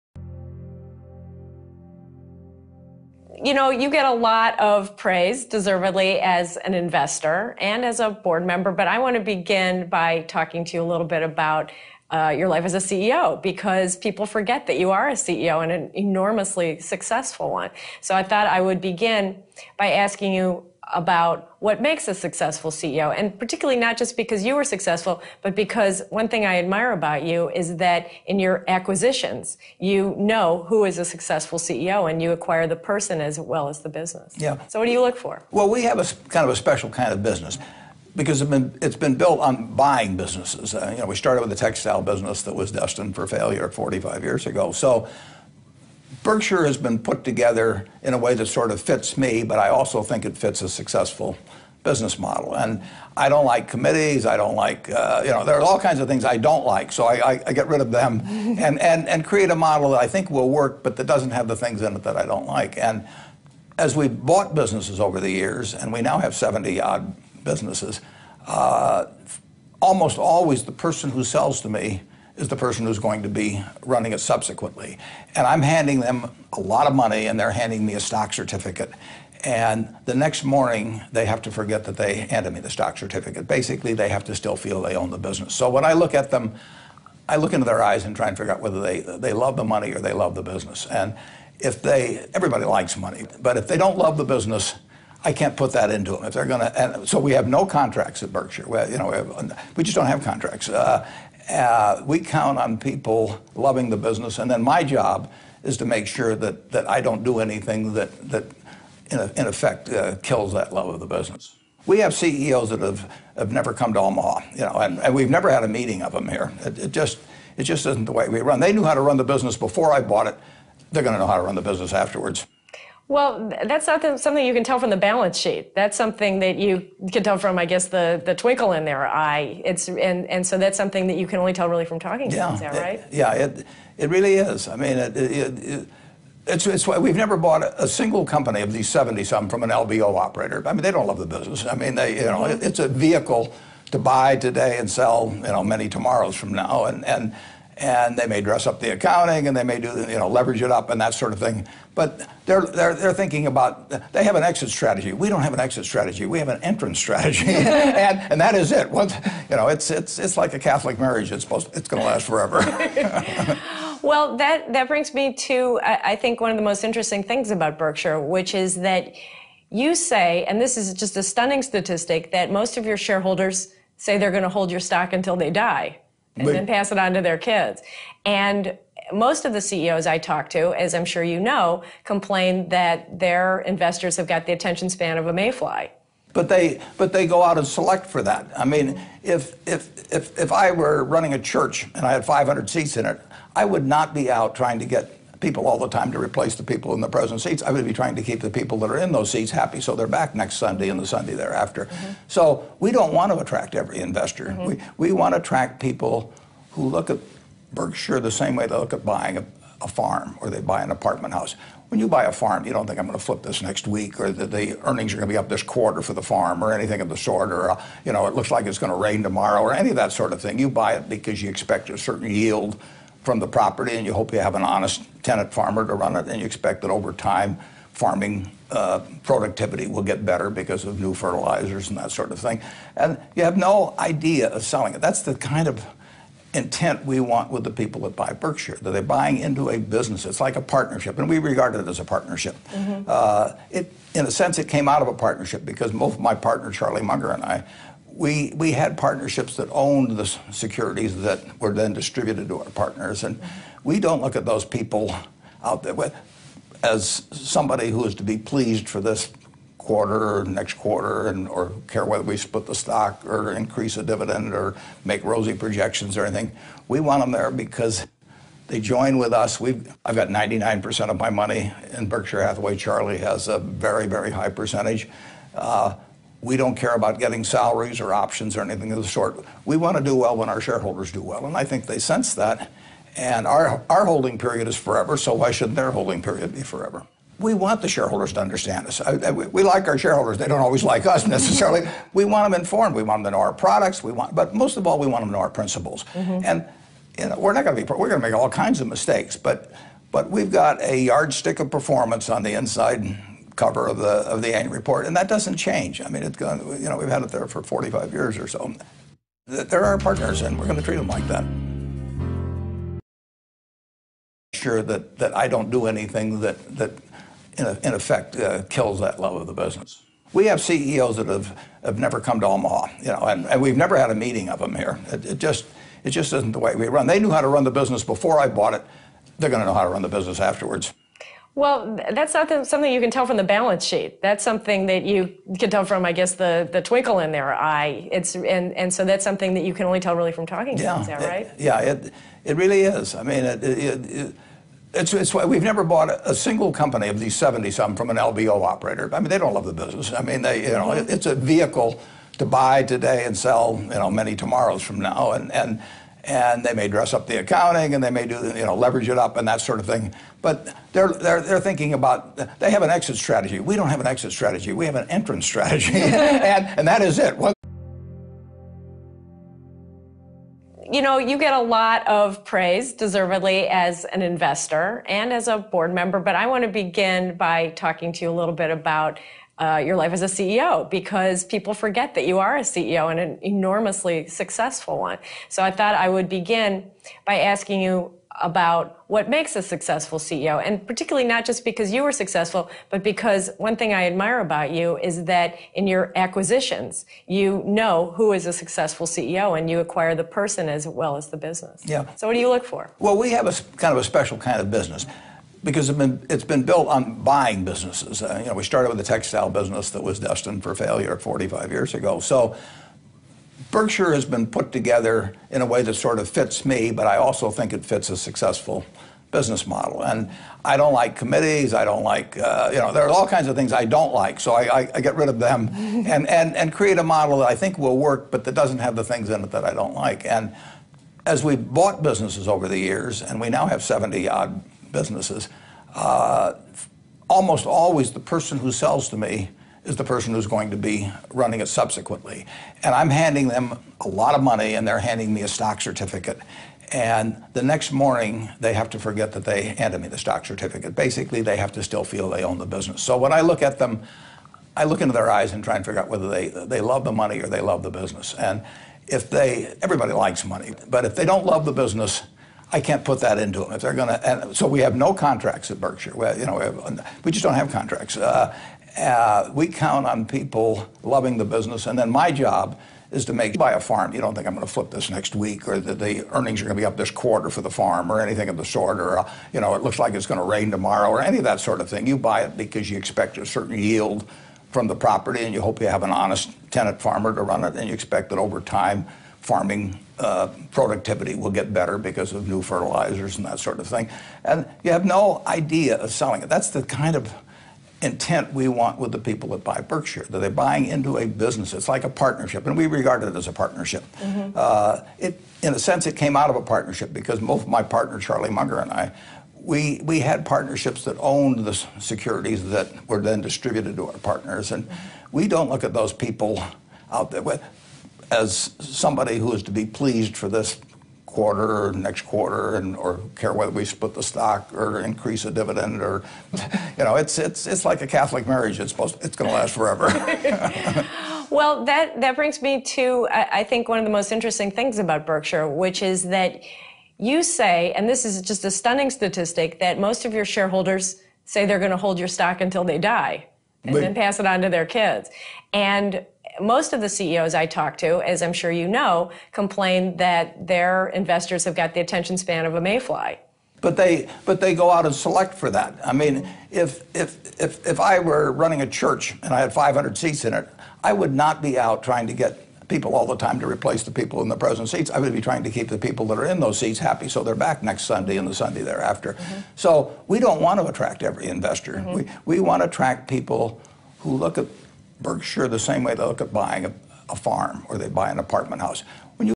Warren Buffett-Berkshire Hathaway's Philosophy
Warren Buffett Discussing his Way of picking CEO's and investors. Books about Warren Buffett and his favourite books are located at the bottom of the